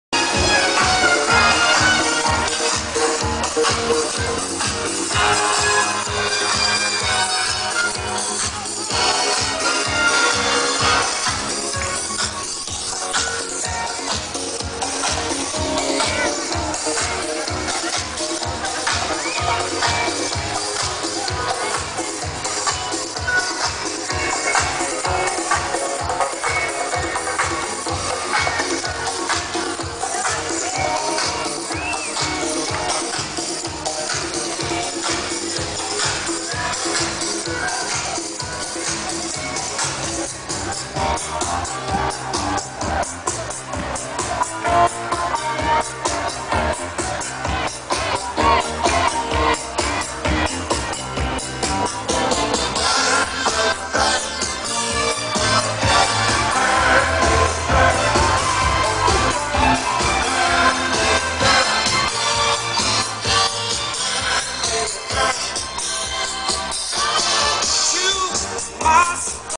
Bull Crazy Music
Boda el 2 de Mayo en la quinta Neiba...un fragmeto de musica de los  70´S